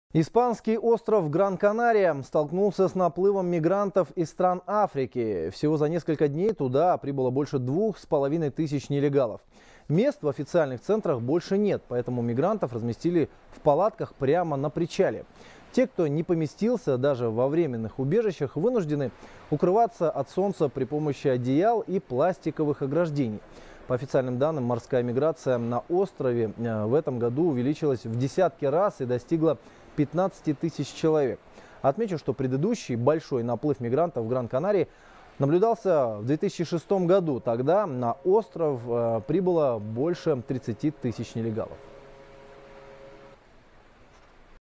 Больше 2,5 тысяч мигрантов прибыло на испанский остров Гран-Канария
Испанский остров Гран-Канария столкнулся с наплывом мигрантов из стран Африки. Всего за несколько дней туда прибыло больше двух с половиной тысяч нелегалов, передает «Хабар 24».